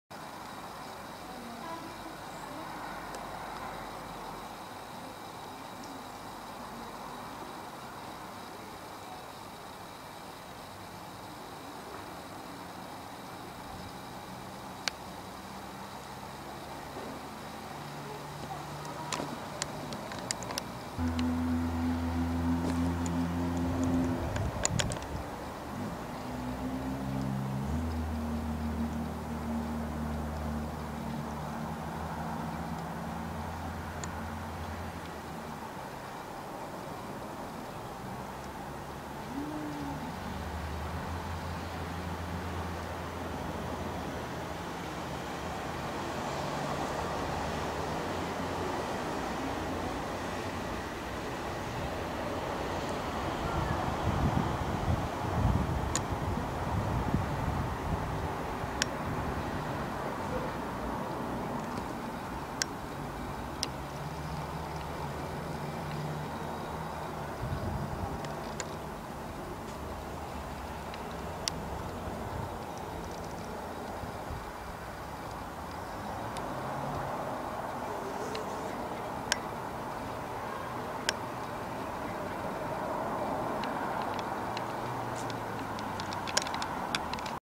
Ants eat fig Then came the fly | ants vs fly
Ants eat fig Then came the fly
Ants food and then comes the more complicated fly with ants , see how fast Serono ant gets upset